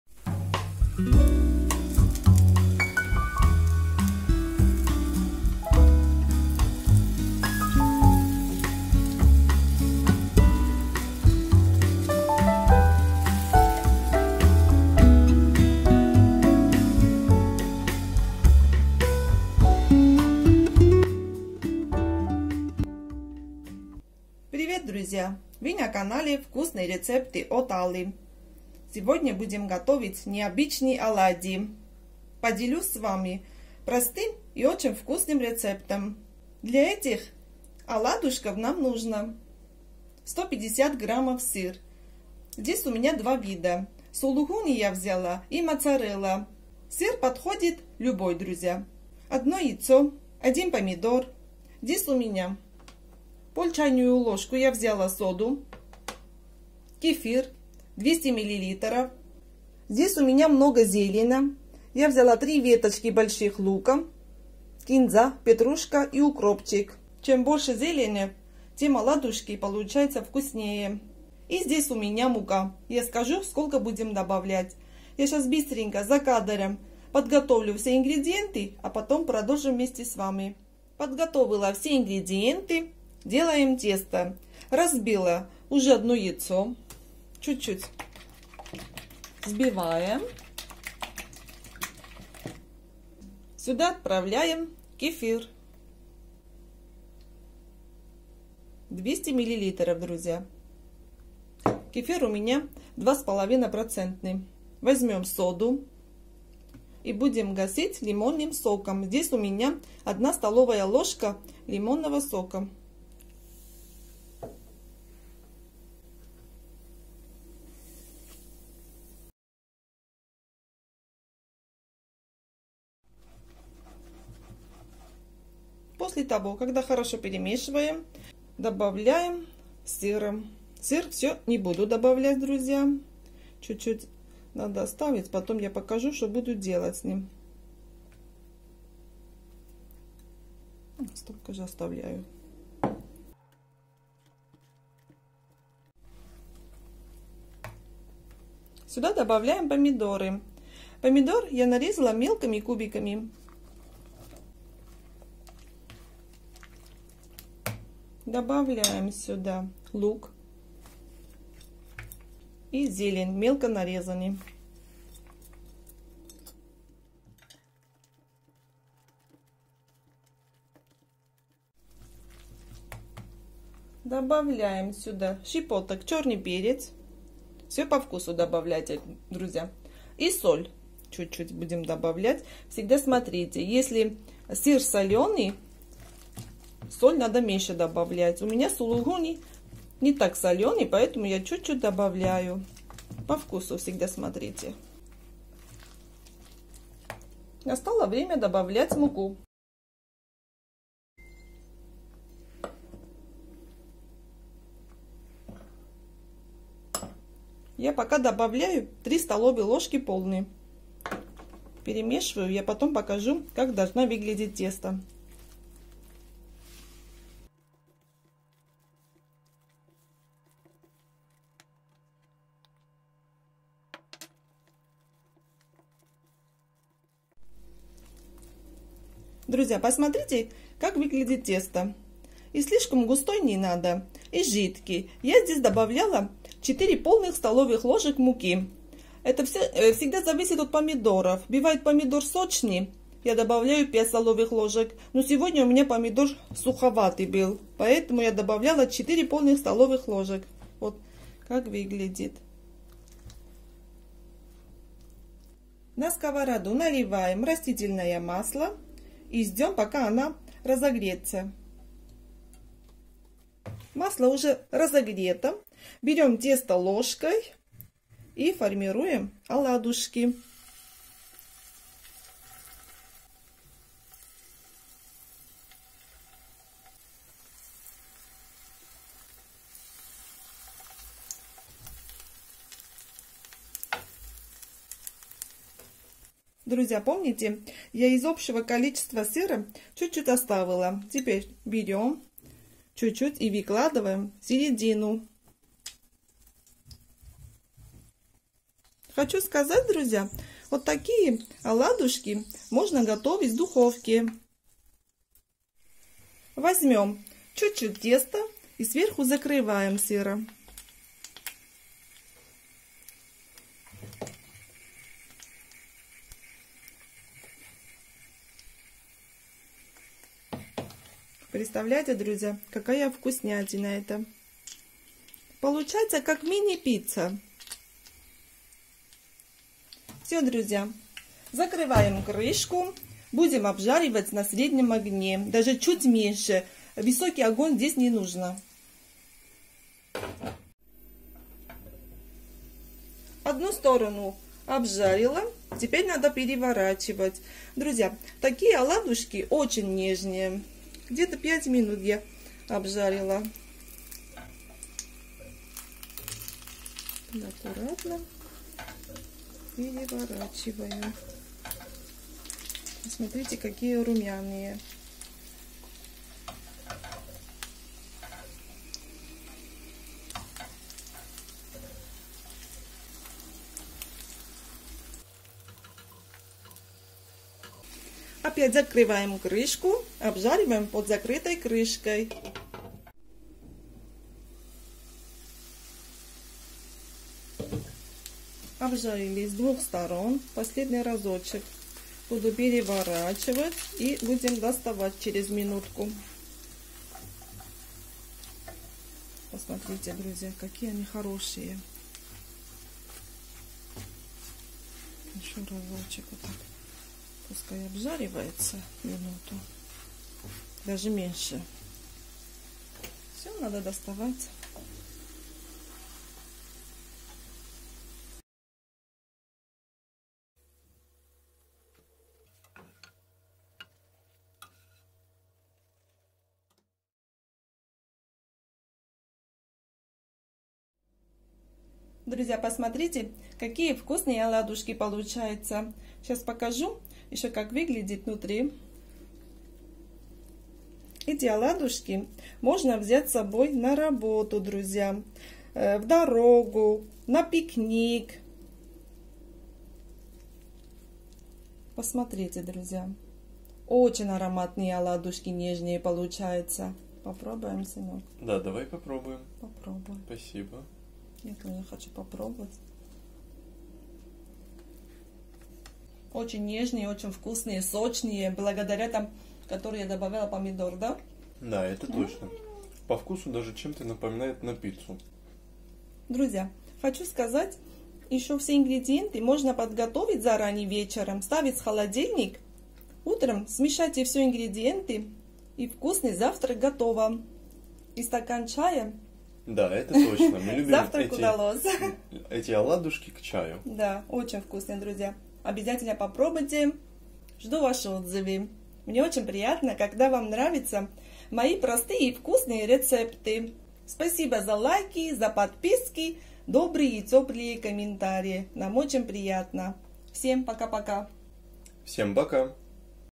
ВКУСНЯТИНА ИЗ ПРОСТЫХ ПРОДУКТОВ! РЕЦЕПТ БОМБА!
РЕЦЕПТ
Кефир 2,5% - 200 мл
Мука - 4-5 ст л
Сыр - 150 г (моцарелла, сулугуни, но так же можно любой по вкусу)
Сода - 0,5 ч л  Гасить 1 ст л лимонного сока
Зеленый лук - 3-4 веточки
Зелень ассорти